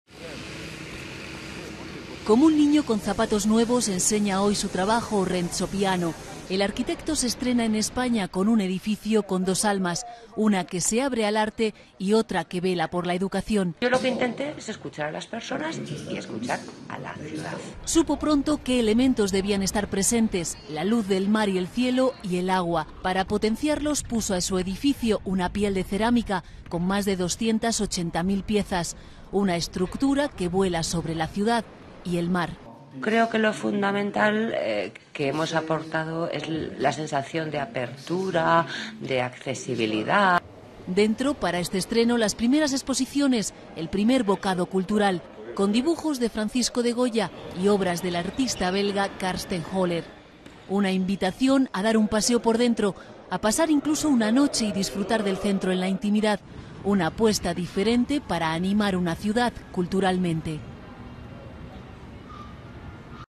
Renzo Piano presenta el Centro Botín en Santander
En relation avec l'article "Santander se ha puesto de moda" en page 6 du magazine Vocable espagnol n°780